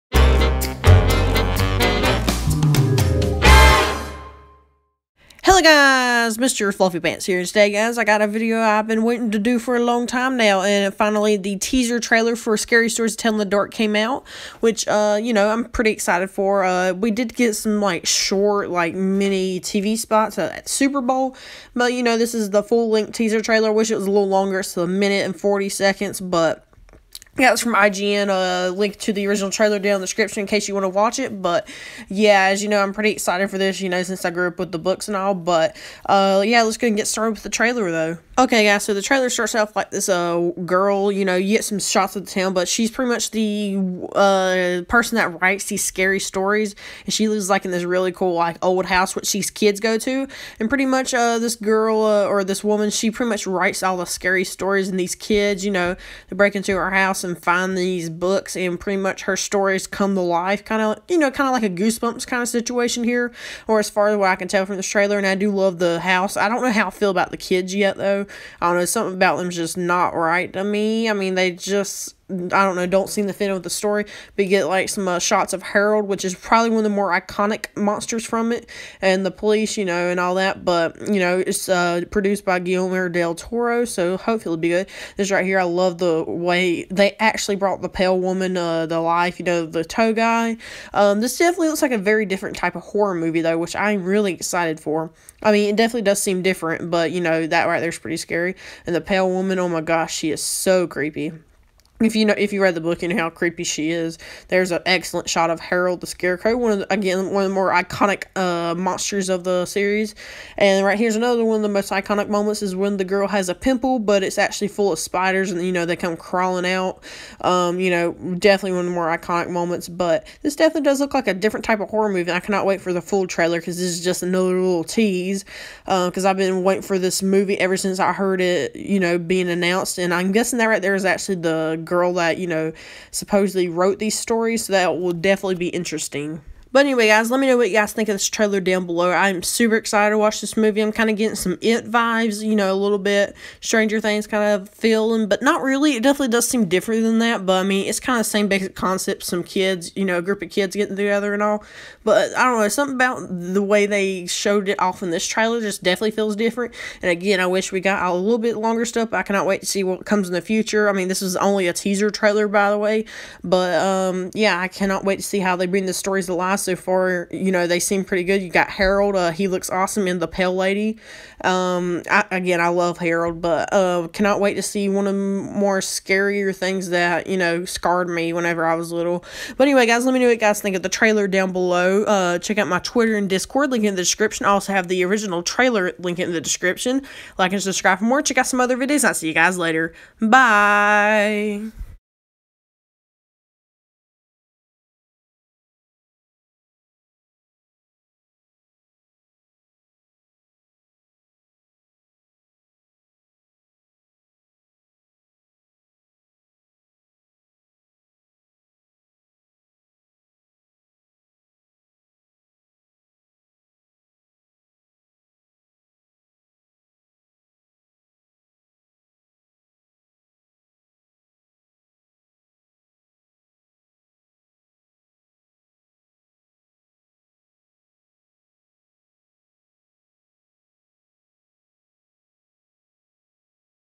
Scary Stories To Tell In The Dark Teaser Trailer Thoughts And Reviews!
Today guys I give my thoughts on my highly anticipated Scary Stories To Tell In The Dark teaser trailer. Let me know what you guys think about this teaser trailer down below!